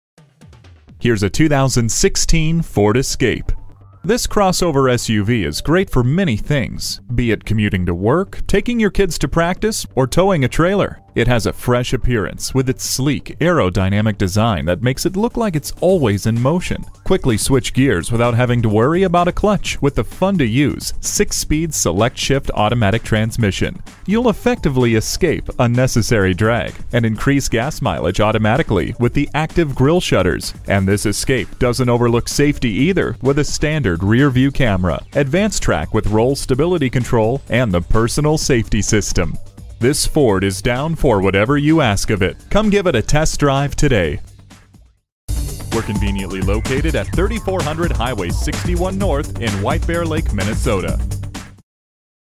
Used 2016 Ford Escape St-Paul White-Bear-Lake, MN W80705A - SOLD
Year: 2016
Make: Ford
Model: Escape
Trim: 4WD 4dr SE 1.6l 4cyl
Engine: 1.6 L 4 Cyl. Cyl.
Transmission: Automatic
Color: Ingot Silver
Interior: Charcoal Black
Mileage: 20670
Stock #: W80705A
VIN: 1FMCU9GX3GUB23945
This Escape is a one owner AWD with a clean history report showing no accidents. Never a rental. Equipped nicely with Cruise, power windows and locks, Bluetooth, alloy wheels, all wheel drive and more, this is a perfect SUV to replace your current ride. You will be impressed by how well taken care of this one is.

Address:
3400 Highway 61 N